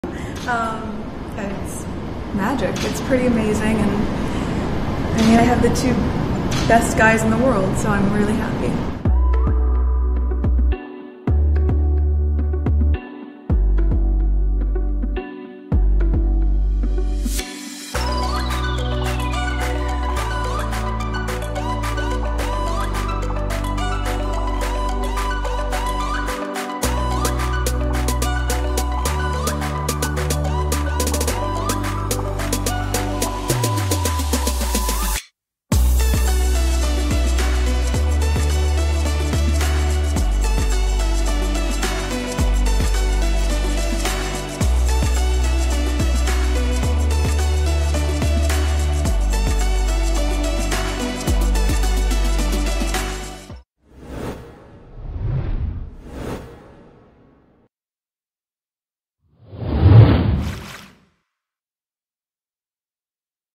يدعى المولود الملكي أرشي هاريسون مونتباتن-ويندسور
يدعى المولود الملكي أرشي هاريسون مونتباتن-ويندسور. ليس لديه لقب نبيل وقد يكون مضطرًا لدفع ضرائب للولايات المتحدة! 👶🏻👑